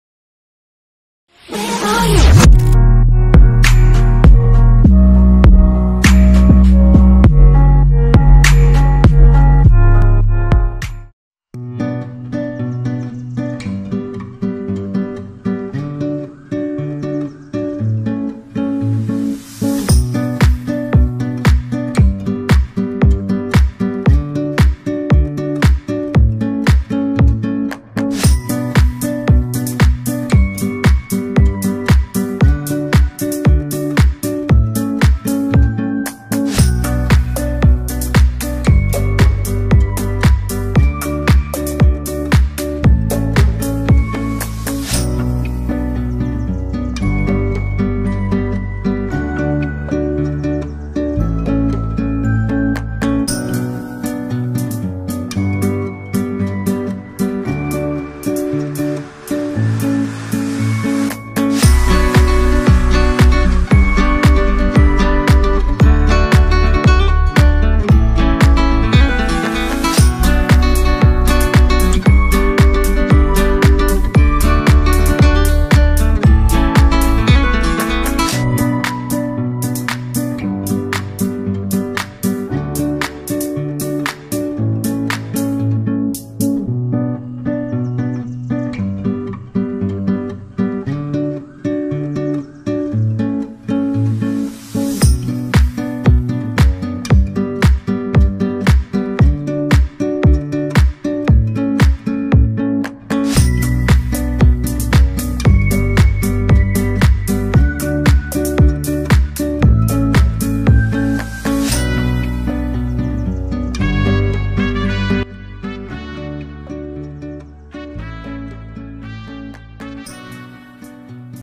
HOW TO SETUP NETWORK NVR / DVR || HIKVISION NVR
In This Video We Show You How to Configure HIKVISION DVR/NVR dns Remote View Setup Step By Step.